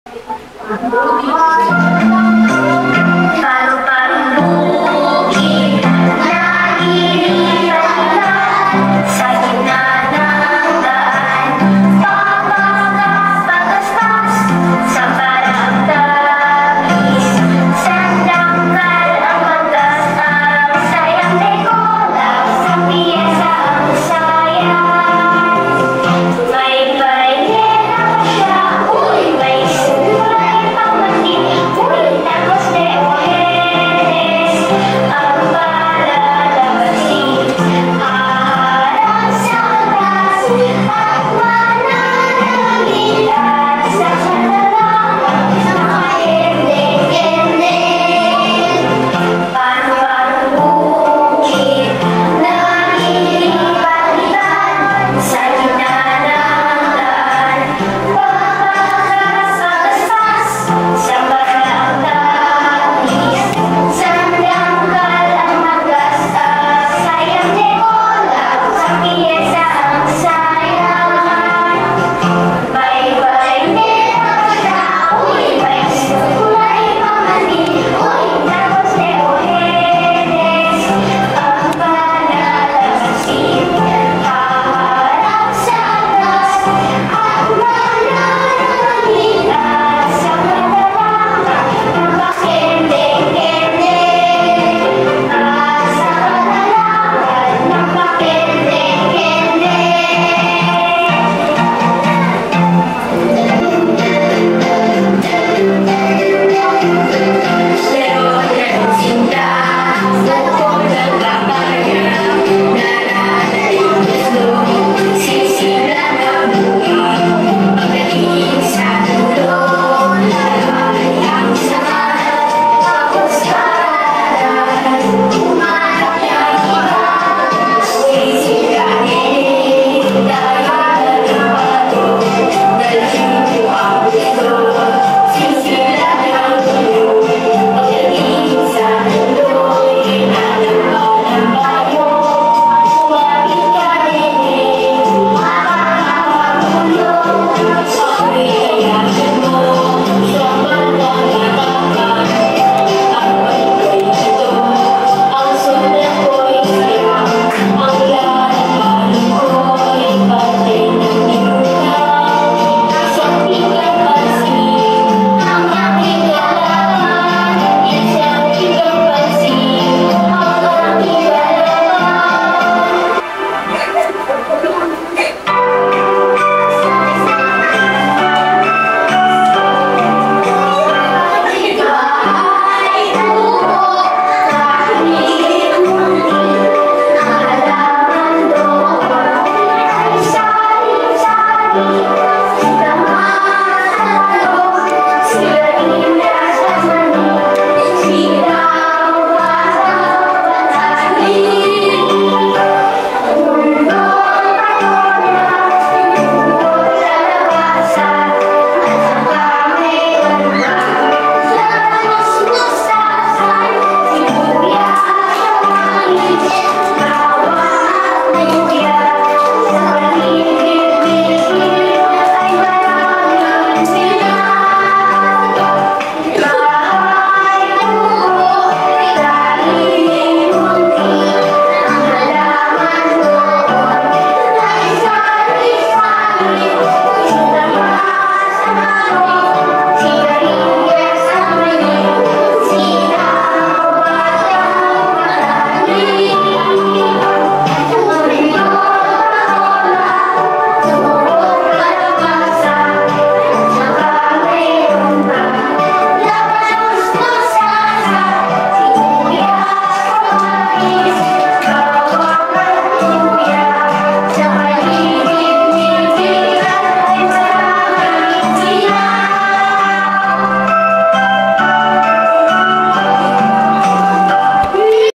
Performance task in Music l Singing Folksongs with action l Rutch TV
Ang mga bata ay nagpakita ng kanilang perfromances sa pamamagitan ng pagkanta ng mga awiting pambata na may galaw